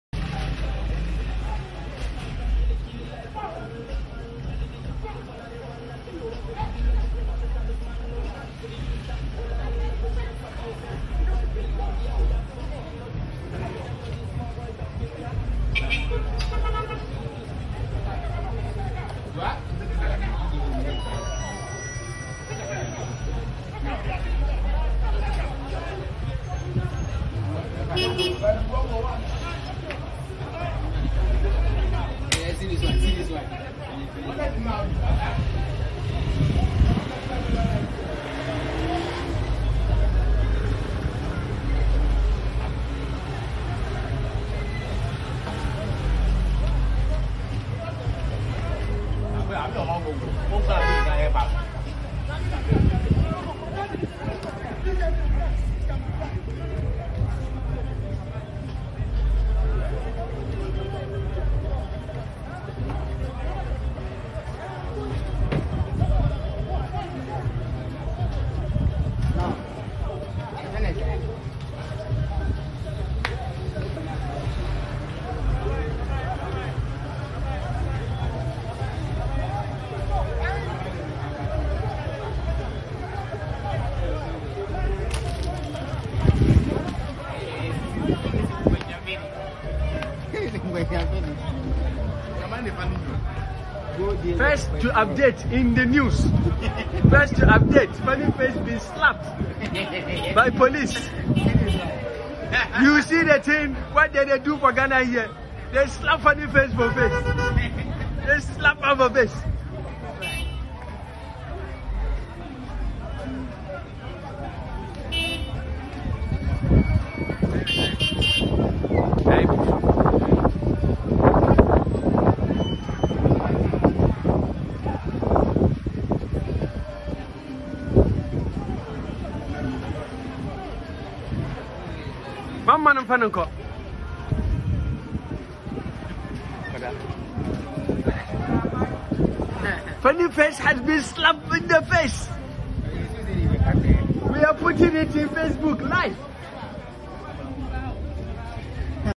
Sad Video of Funny Face Brutalised by Ghana ....
Please scribe to our channel ...
From Funny Face "GHANA POLICE BURN IN HELL 😡😡😡🔥🔥🔥"
GHANA POLICE .. mo maaaame tw3 .. armed robbers will continue to shoot and kill u all in JESUS name ✊🥵 Amen 😡😡😡😡🔥🔥🔥🔥🔥 STOPPOLICEBRUTALITY 🔥🔥😡😡😡😡